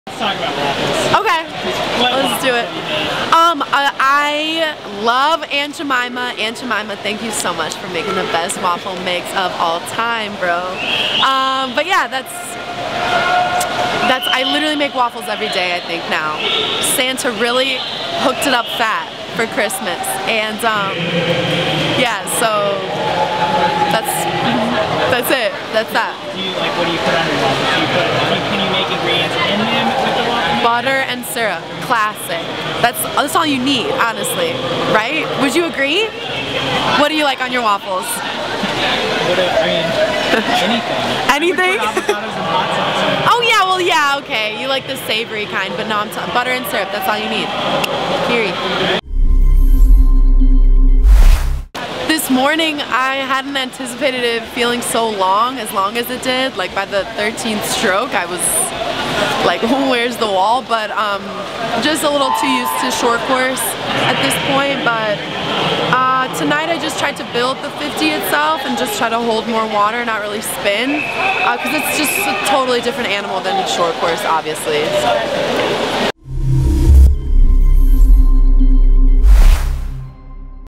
Olivia Smoliga is Stoked About her New Waffle Maker
Olivia Smoliga, coming off of a record 8 gold medals at World Championships (SCM) in Hangzhou, is all smiles. But not because of the medals. As she puts it, "Santa really hooked it up fat for Christmas."